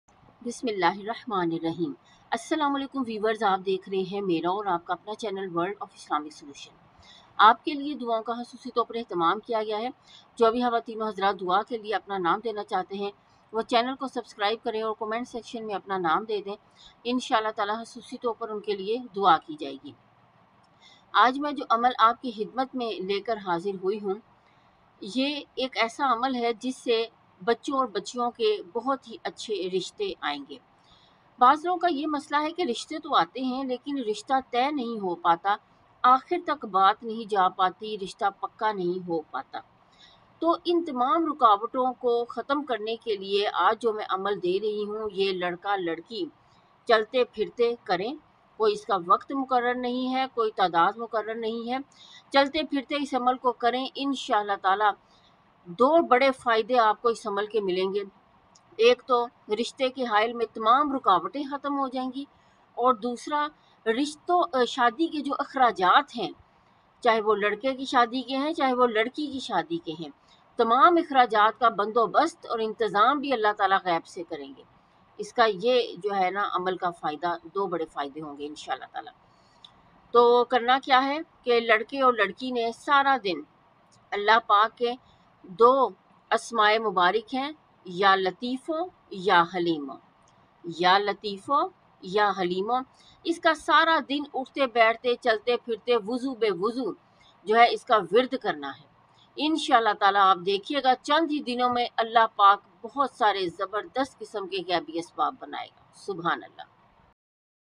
Amal for Urgent Marriage and to Bear Expence of Marriage
@worldofislamicsolution2807 In this video amal of ya Latifo ya Haleemo is explained to get married urgently and bear expence of marriage.